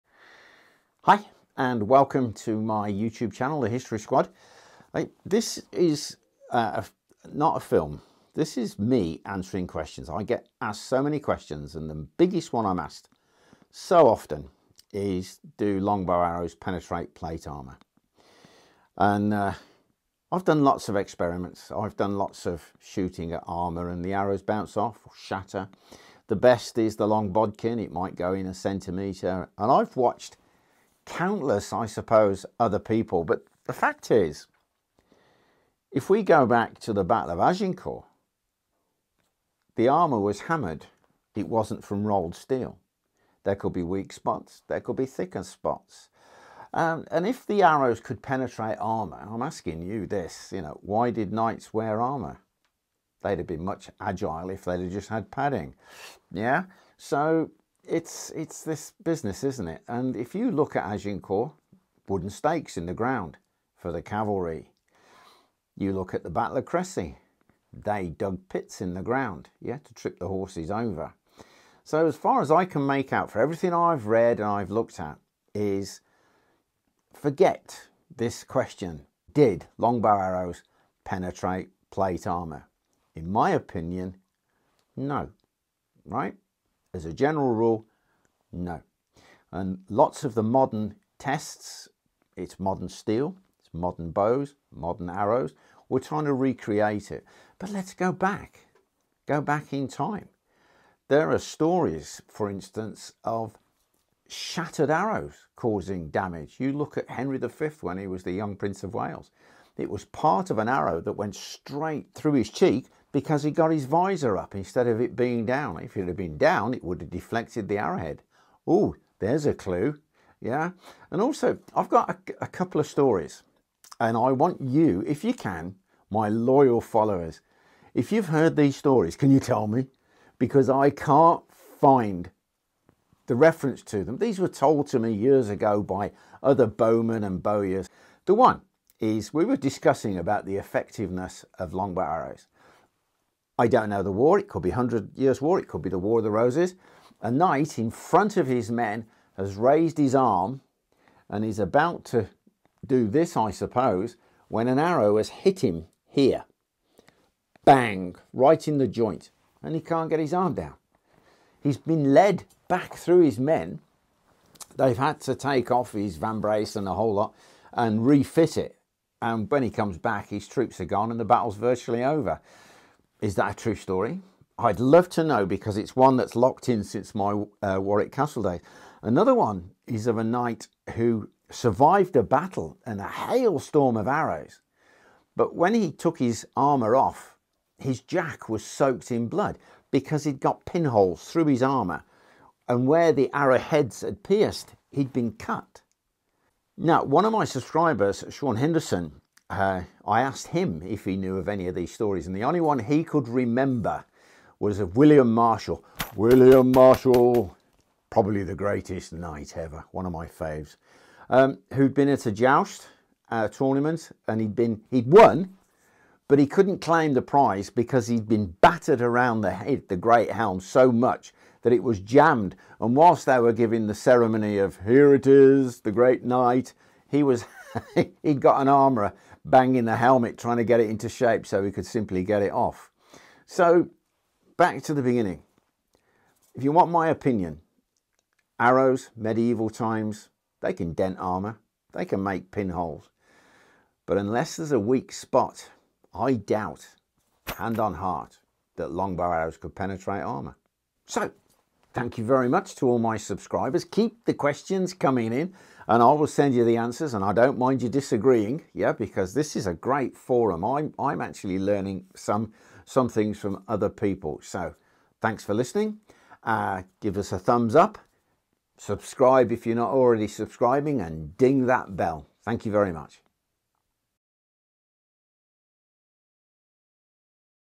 Could medieval longbow arrows penetrate plate armour?
Welcome to another Q&A session about history, the longbow, archery or whatever subscribers want to know.  Can longbow arrows penetrate plate armour?  It's a question that's asked a lot, in varying ways, so here, Kevin Hicks shares his experience and thoughts on the question.